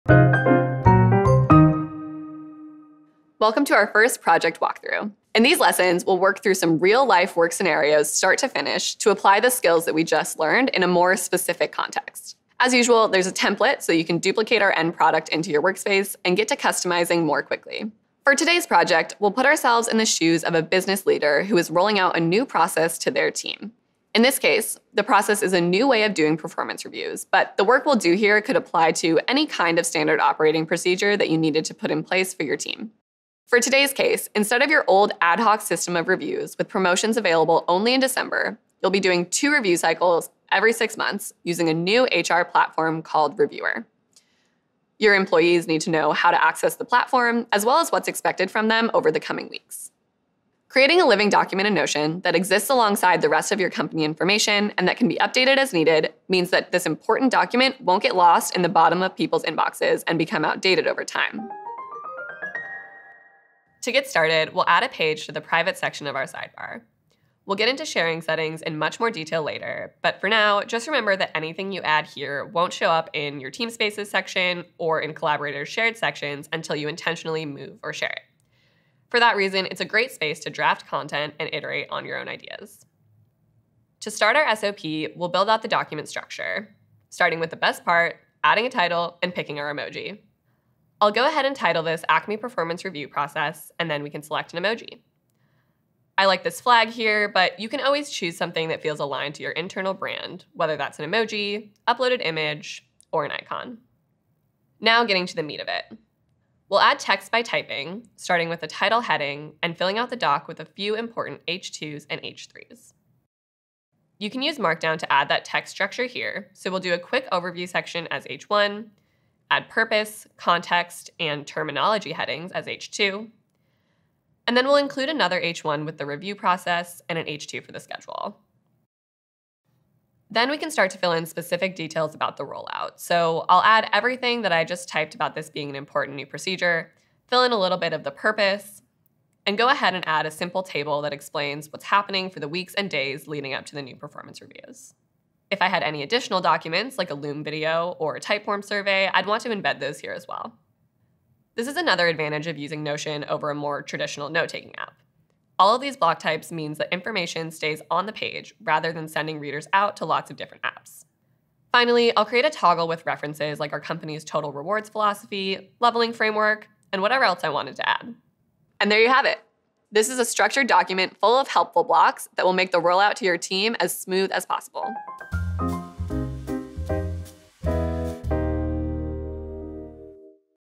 How to build a standard operating procedure doc
Notion 101 (part 3 of 12). Apply your new knowledge about blocks and editing to build a standard operating procedure doc for your team.